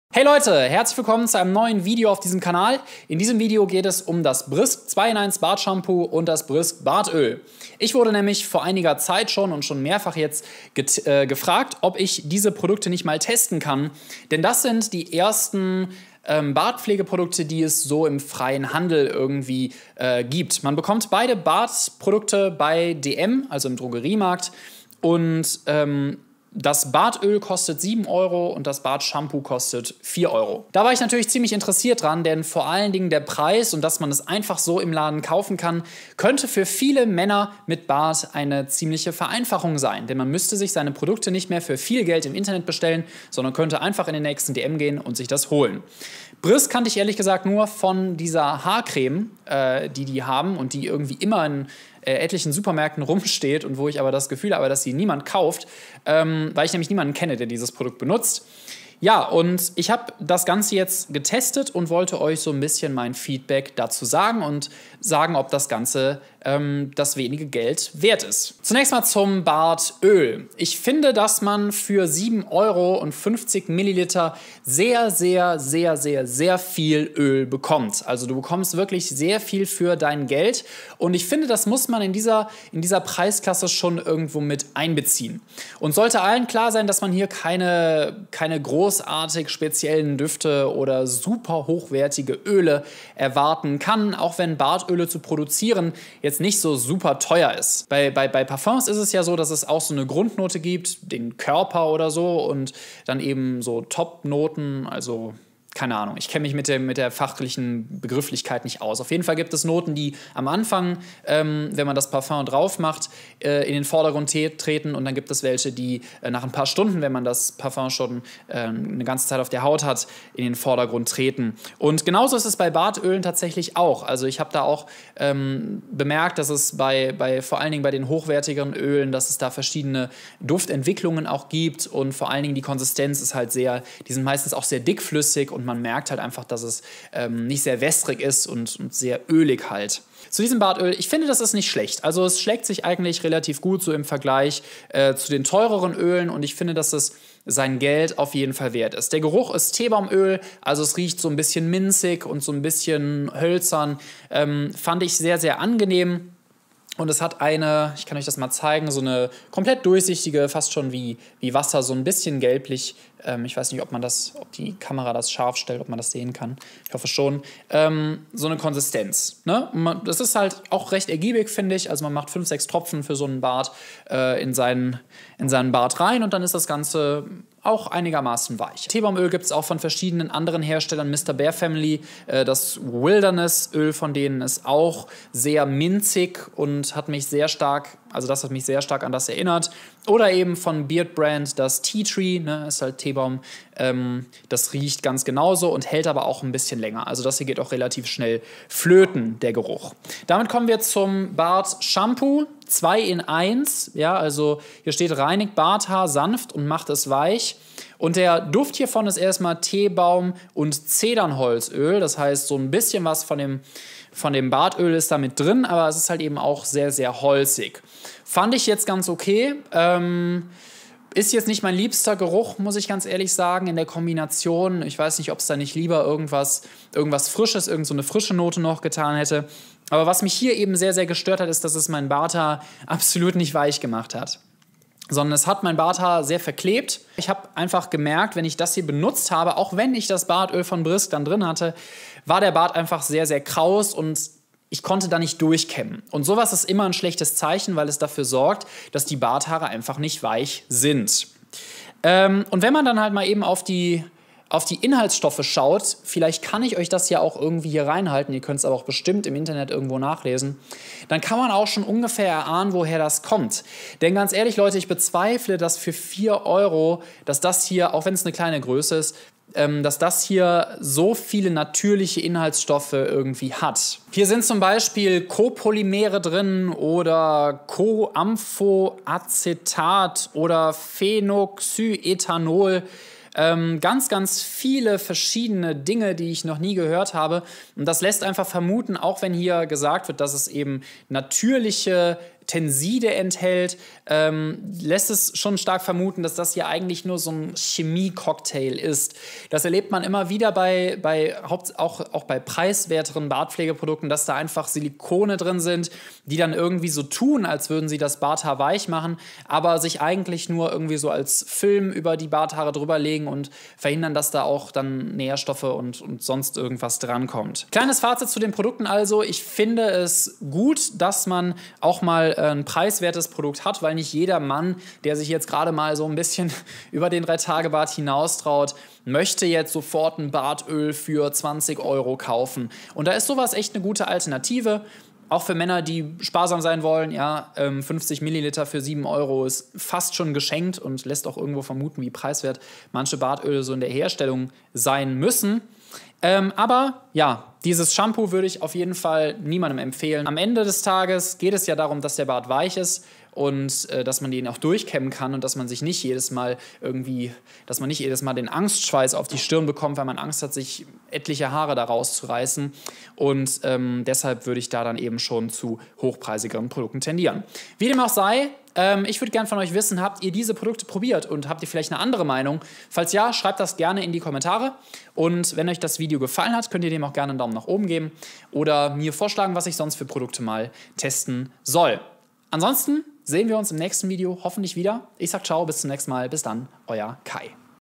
Brisk Bartöl & Shampoo Review | Bartpflege zum Niedrigpreis? | Kai vloggt
➡ Mein Bart-Trimmer